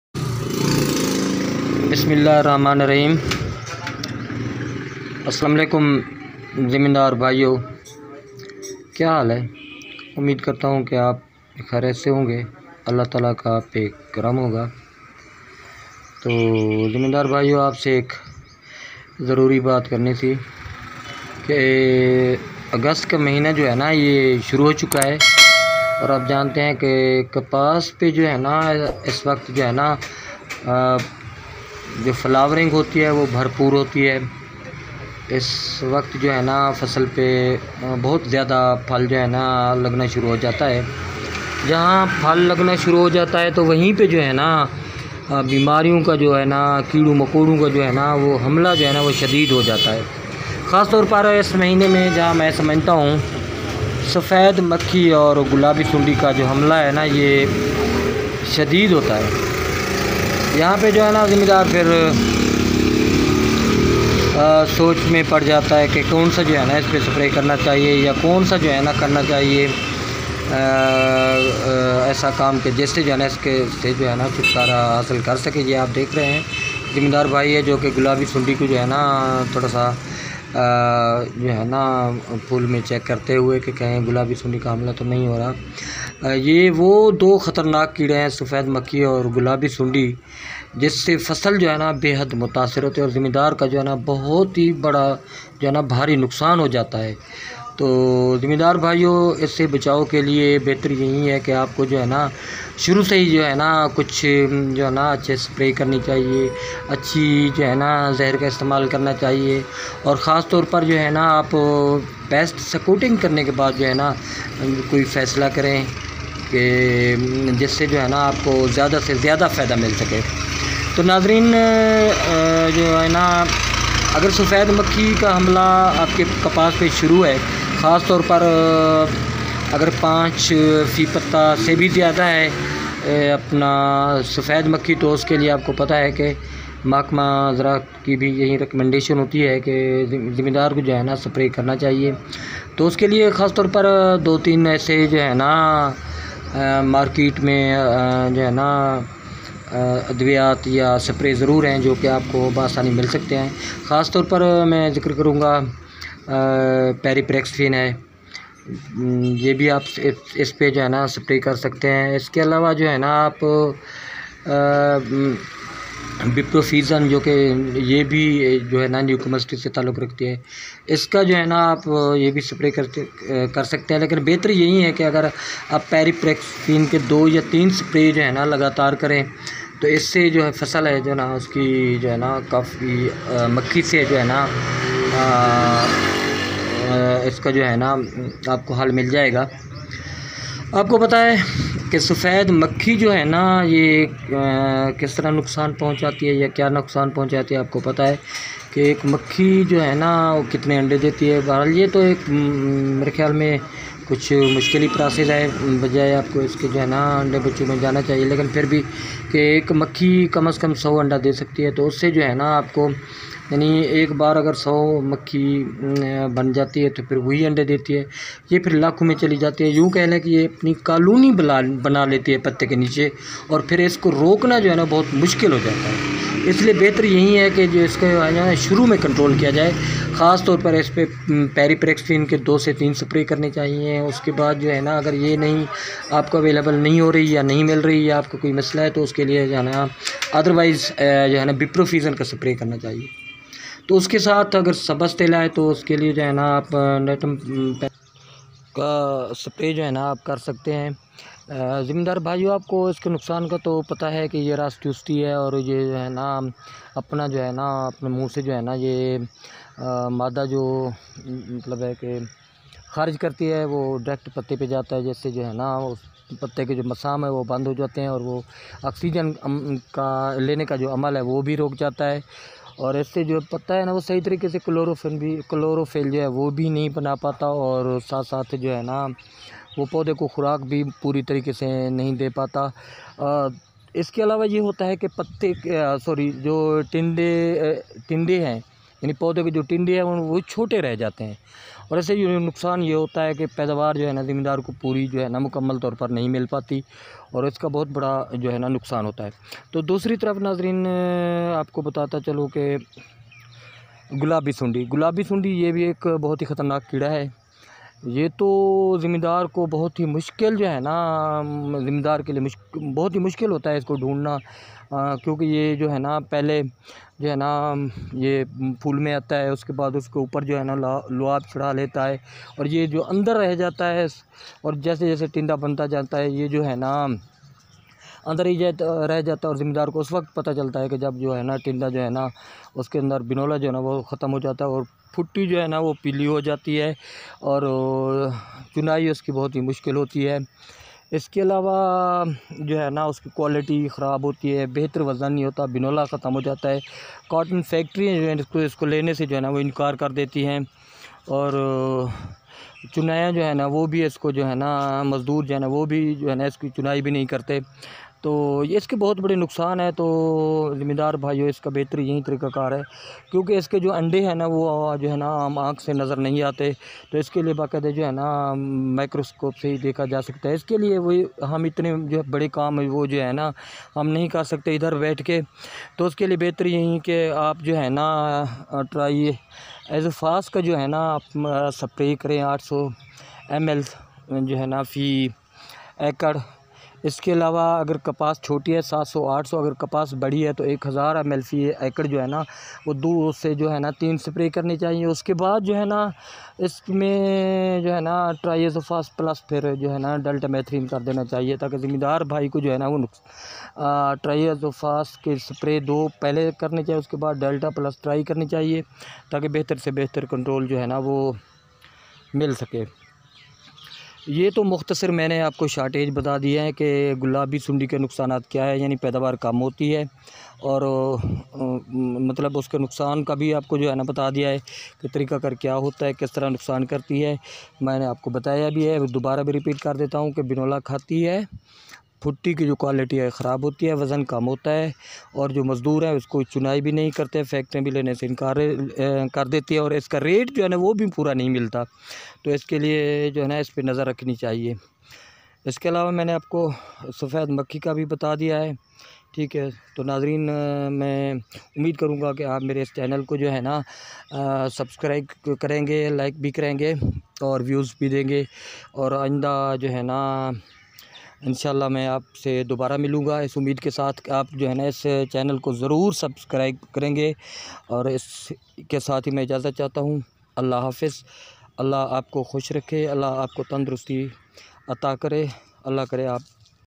How To Control White fly and Pink bollworm in Cotton Crop|Gulabi Sundi Aur Sufaid Makhi Ka Ilaj 2020
How To Control Whitefly and Pinkbollworm in Cotton Crop|Gulabi Sundi Aur Sufaid Makhi Ka Ilaj 2020